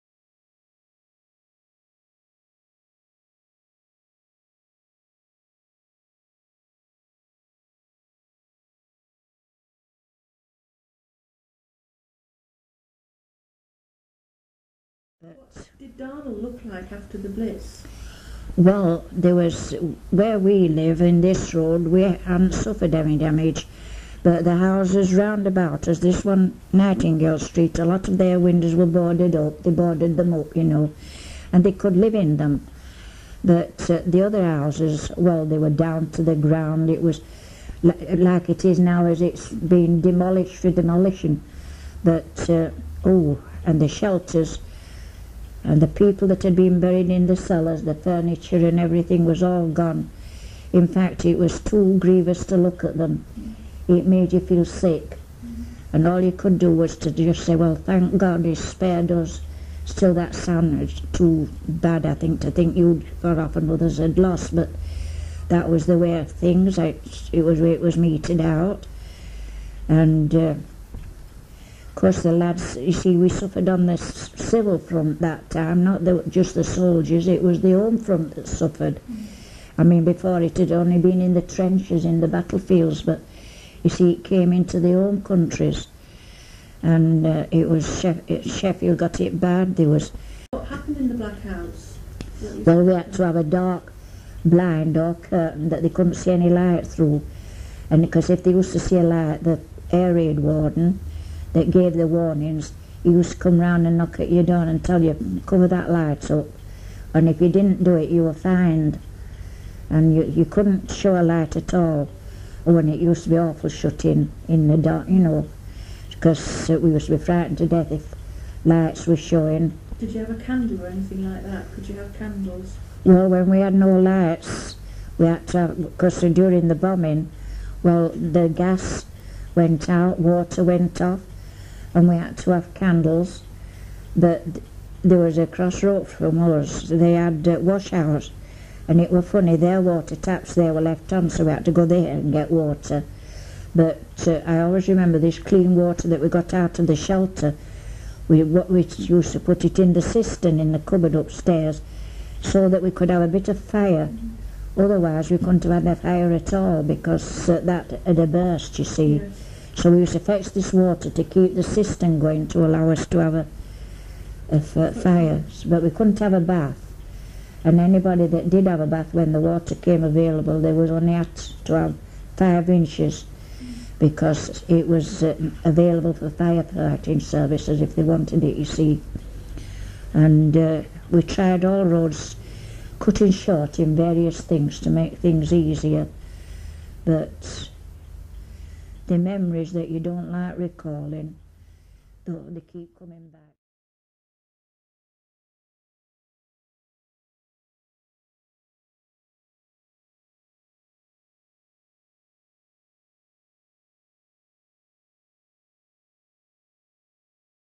Women of the Sheffield Blitz - Florence Askham - Part 2
Florence Askham, speaking in the 1980s, recalls living in Sheffield during the Blitz of 1940.